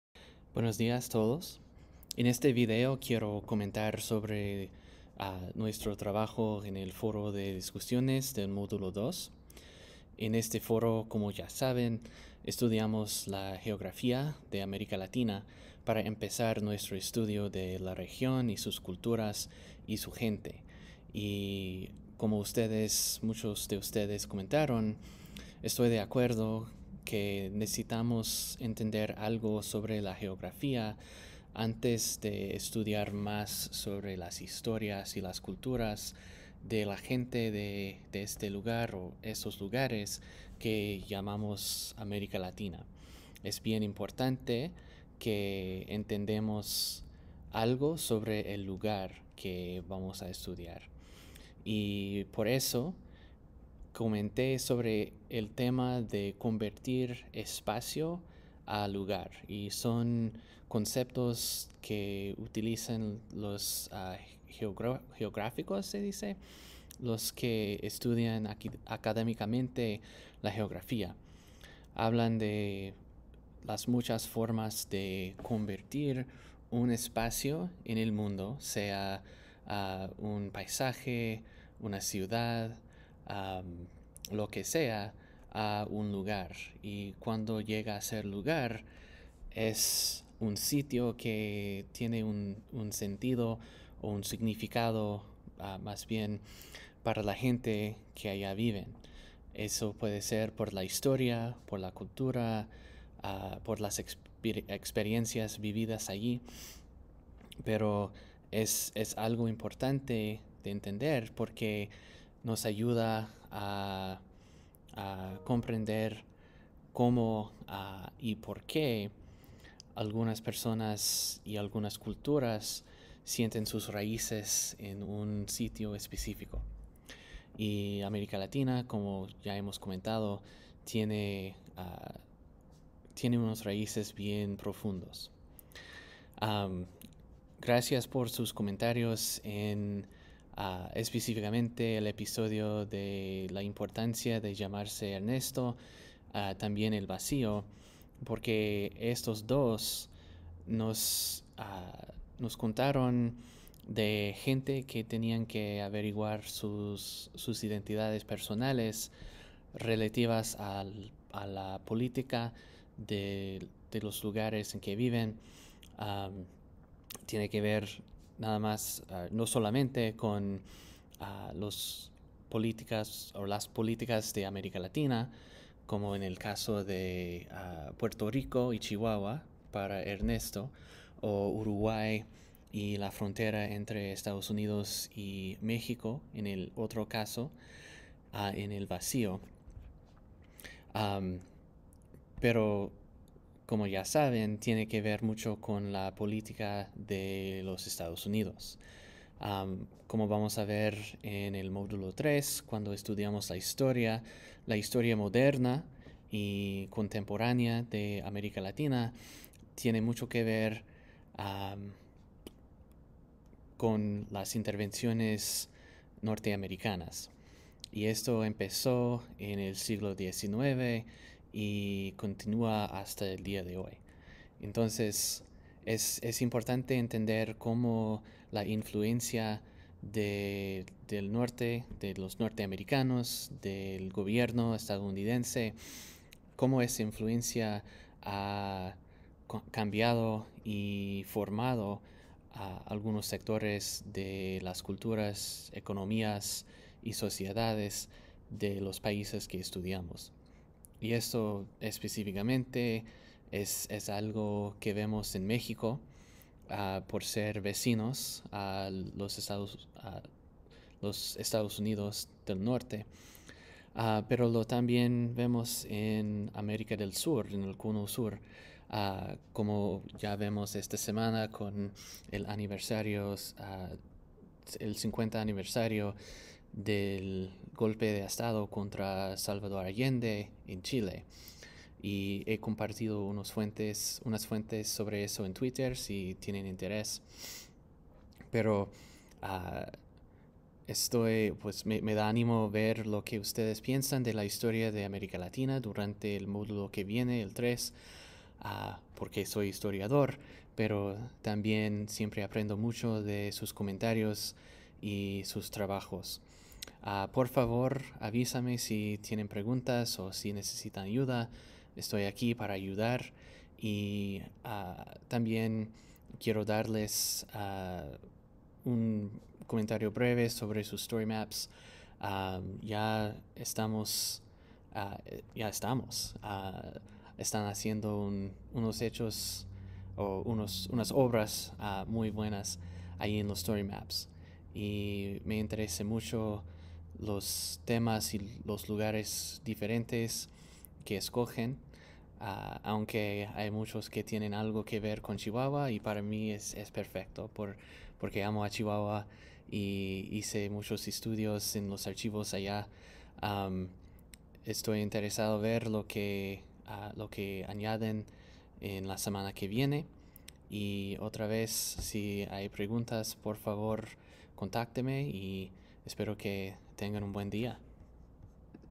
Comentarios sobre Mod 2, 1110 español
En el cual doy unos comentarios sobre nuestro trabajo durante Módulo 2